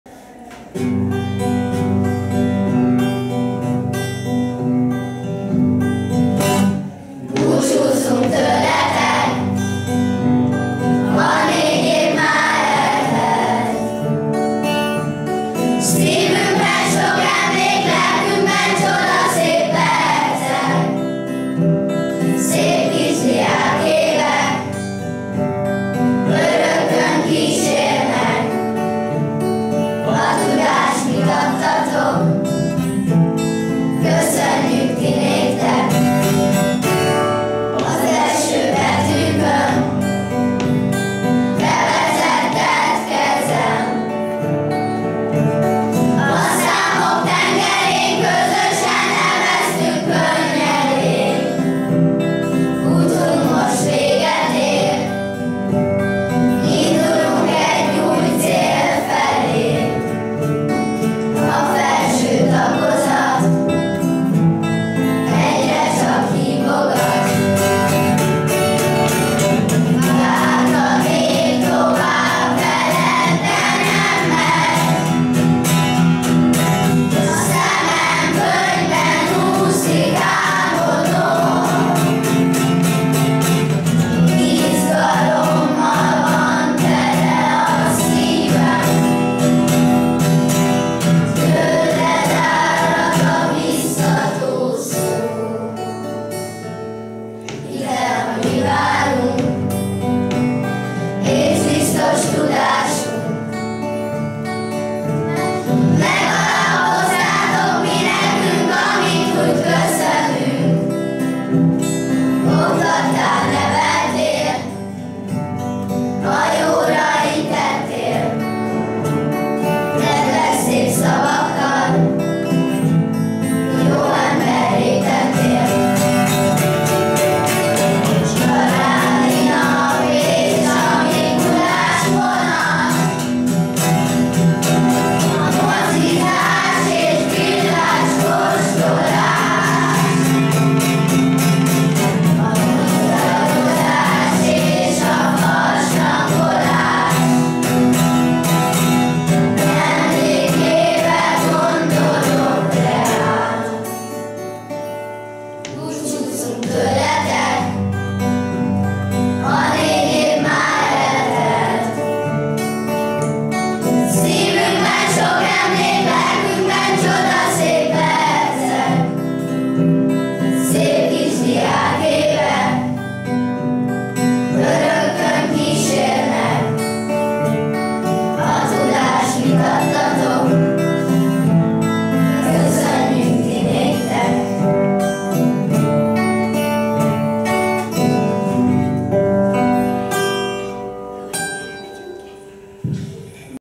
Búcsúdal
A Lajosmizsei 4.b  osztály búcsú  éneke a Tanítóiktól 2019ben!